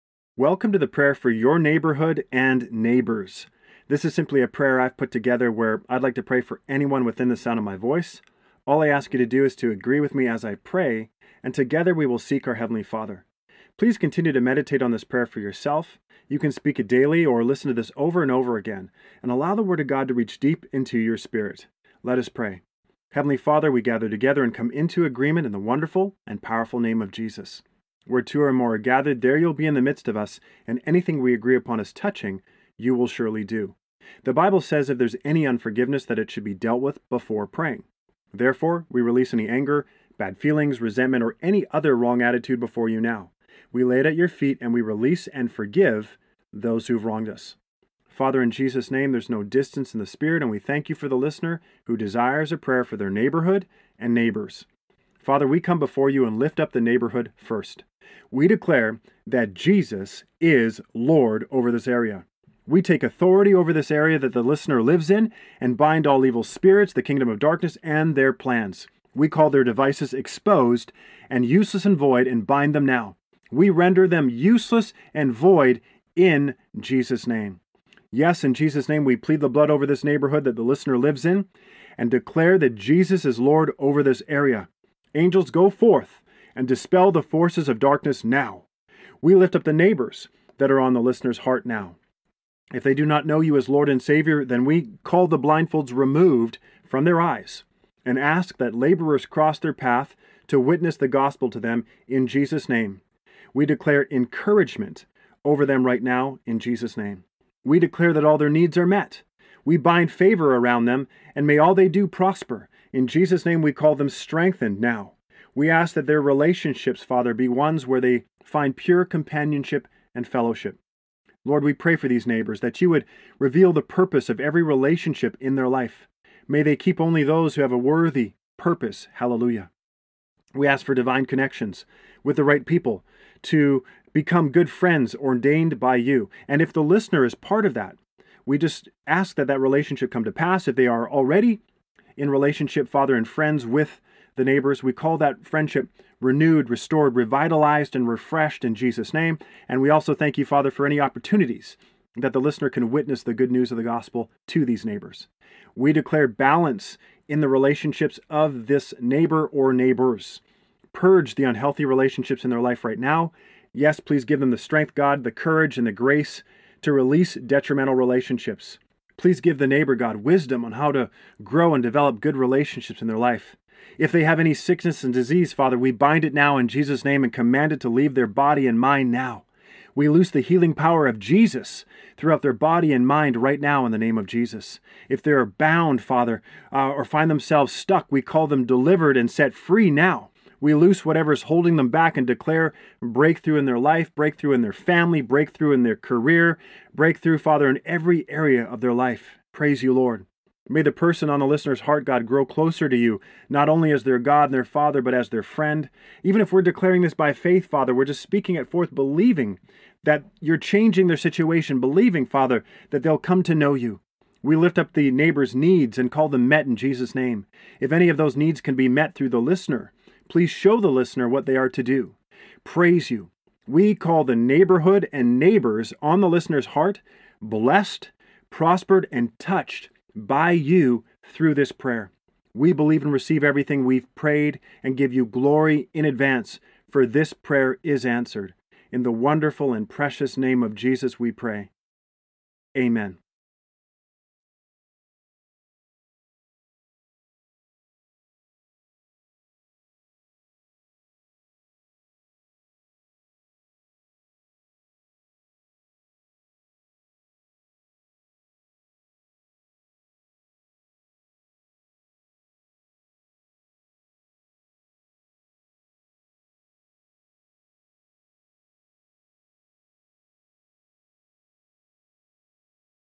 Prayer For Neighborhood and Neighbors | Neighbourhood Cleansing Prayer
All I ask you to do is to agree with me as I pray and we seek our Heavenly Father regarding prayers for neighbors and neighborhood.

Speak it daily or listen to this video over and over again and allow the Word of God concerning this prayer for neighbours to reach deep into your spirit.